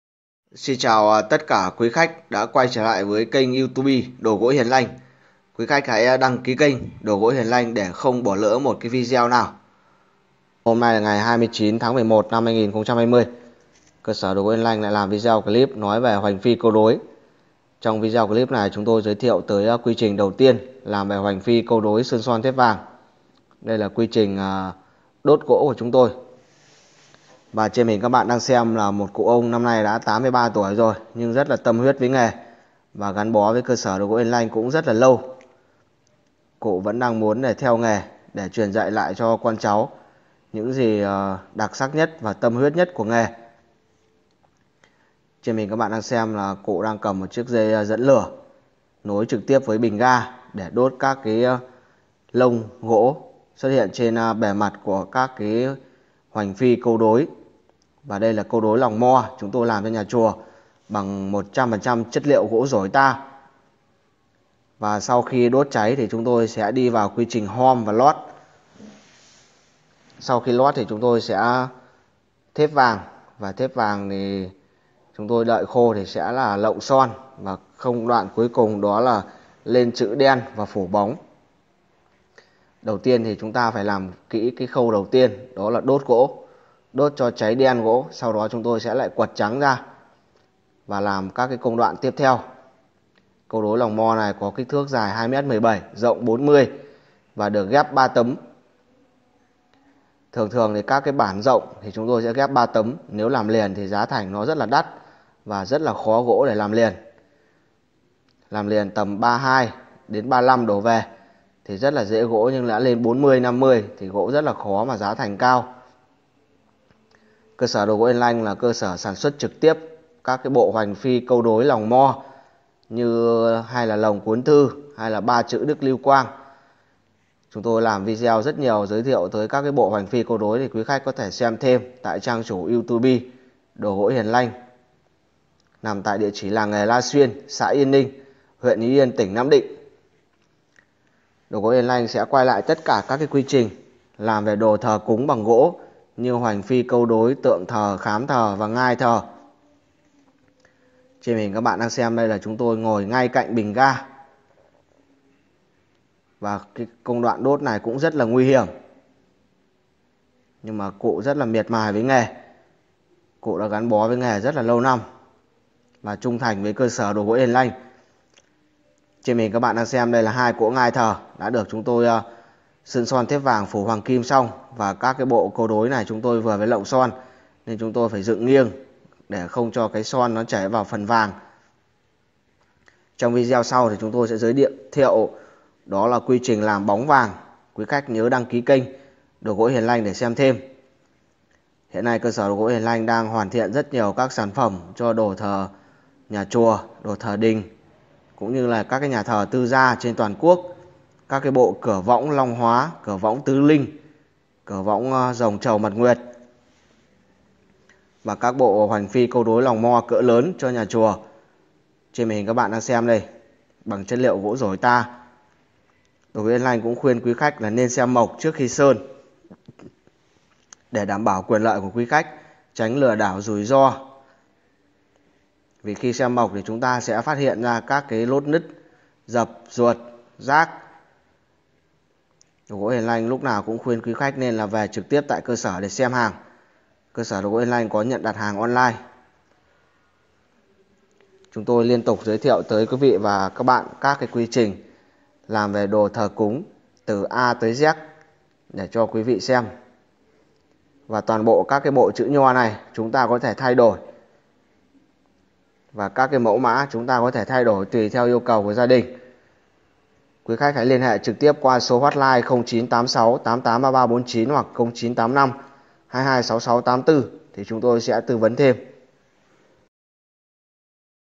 🔴Đốt Hoành Phi Câu Đối gỗ Dổi cực kỳ Nguy Hiểm/ Tại sao? đồ gỗ Hiền Lanh. 0986883349
- 🔴Đốt Hoành Phi Câu Đối gỗ Dổi cực kỳ Nguy Hiểm. đồ gỗ Hiền Lanh. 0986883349
- đốt hoành phi câu đối để làm nhẵn các bề mặt chữ, và các họa tiết hoa văn - sau đó đi vào lót hàng - sơn son thếp vàng - phủ bóng hoành phi câu đối
- 🔴Hoành Phi gỗ gụ có rất nhiều kích thước như sau: dài 1m97 x rộng 61   -   dài 2m17 x rộng 61  -  dài 1m97 x rộng 67   -   dài 2m17  x  rộng 67   -    dài 2m37 x  rộng 81